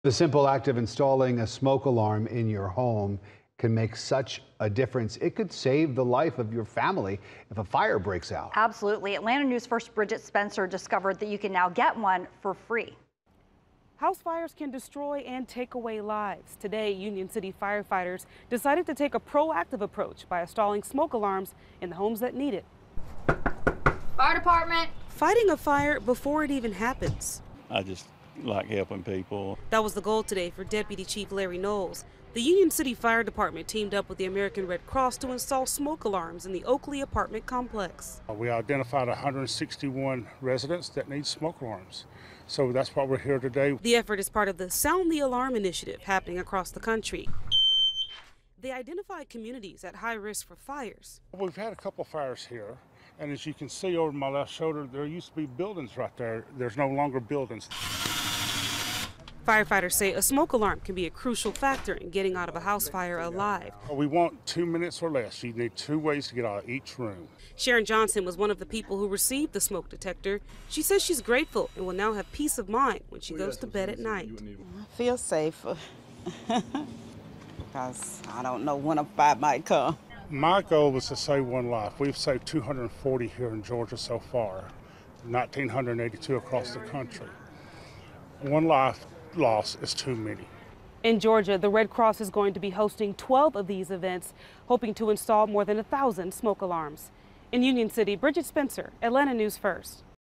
Free smoke alarm installations saving lives in Union City, organizers say
The Union City Fire Department teamed up with The American Red Cross of Georgia to install smoke alarms in The Oakley apartment complex.